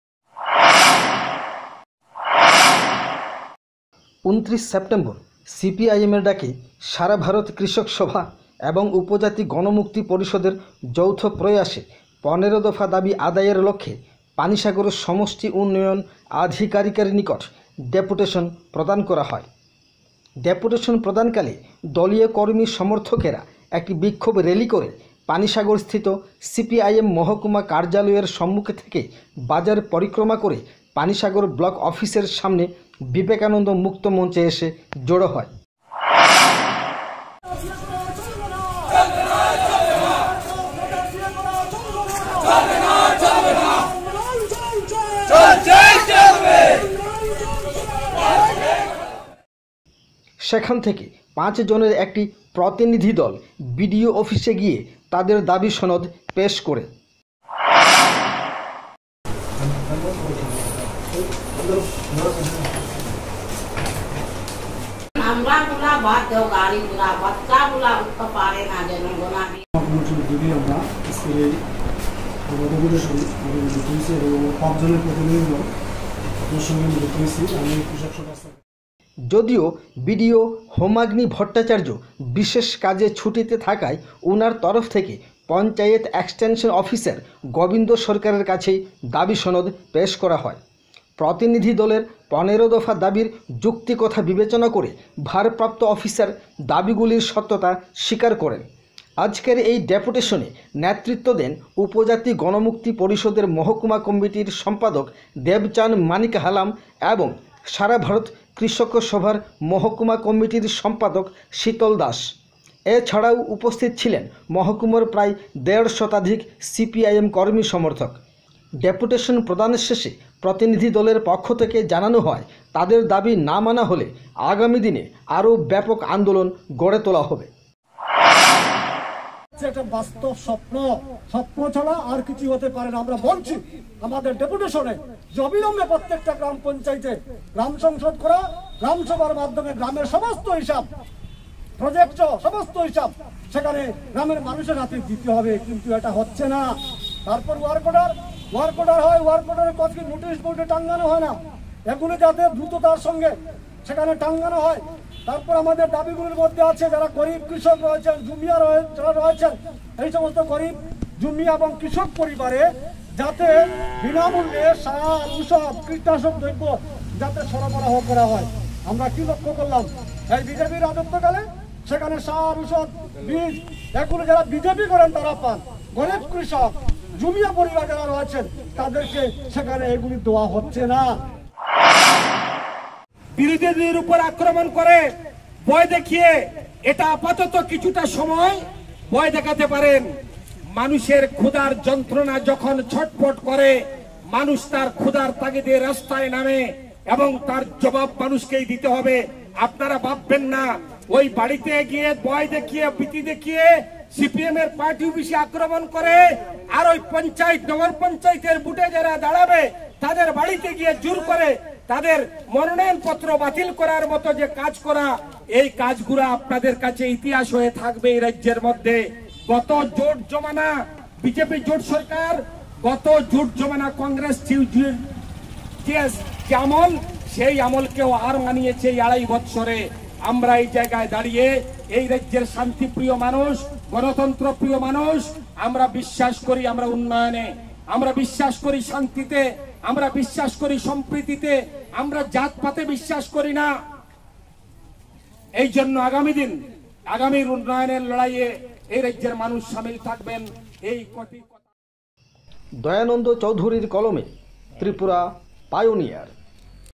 ১৫ দফা দাবিতে সারা ভারত কৃষক সভা ও গণমুক্তি পরিষদের ডেপুটেশন, পানিসাগরে দীপ্ত মিছিল
পানিসাগর সংবাদ